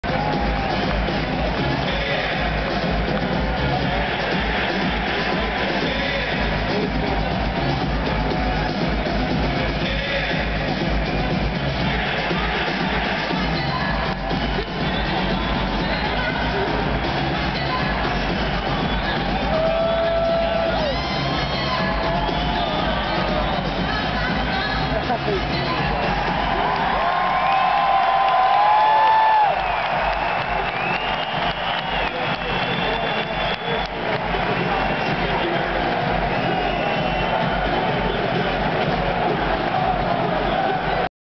Dancin Larry
Larry dancin at the ranger game Feb 24 vs the panthers